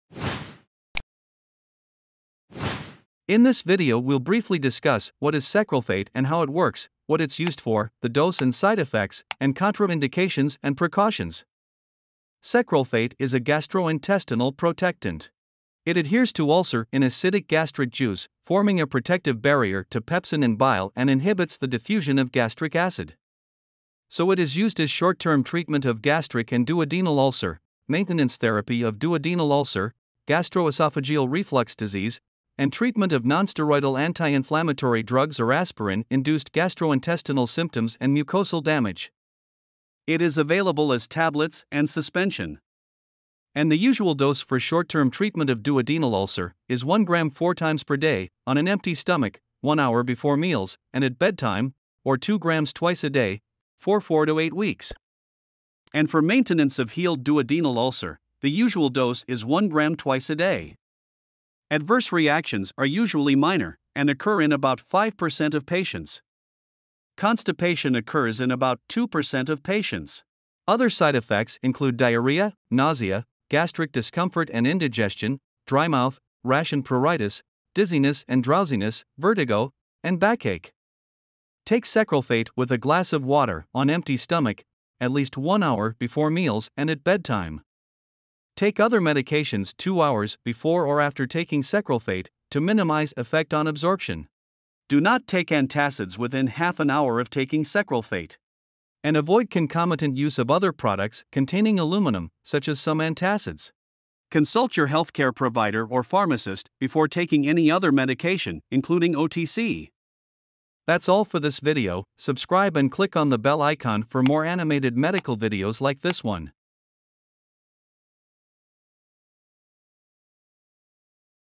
Sucralfate 1 gm (Carafate): What Is Sucralfate Used For? Uses, Dosage and Side Effects of Sucralfate
- This is lesson n# 159 in "A DRUG IN BRIEF" Series. In this video I'm gonna discuss almost all you need to know about Sucralfate 1 gm (Carafate): What Is Sucralfate Used For? Uses, Dosage, Side Effects of Sucralfate, Precautions and Mechanism of Action

- In 3 minutes you'll learn about:

- What is Sucralfate 1 gm (Carafate)?

- Sucralfate is a gastrointestinal protectant. It adheres to ulcer in acidic gastric juice, forming a protective barrier to pepsin and bile and inhibits the diffusion of gastric acid.

- So it is used as short-term treatment of gastric and duodenal ulcer, maintenance therapy of duodenal ulcer, gastroesophageal reflux disease, and treatment of nonsteroidal anti-inflammatory drugs or aspirin induced gastrointestinal symptoms and mucosal damage.

- It is available as tablets and suspension.

- Learn more about Sucralfate 1 gm (Carafate) mechanism of action, uses, dosage, side effects, and precautions in this video...

- Video Chapters:
00:12 - Sucralfate Mechanism of Action
00:23 - Sucralfate Uses
00:39 - Sucralfate Dosage Forms
00:42 - Sucralfate Dosage
01:01 - Sucralfate Side Effects
01:21 - Sucralfate Patient Counseling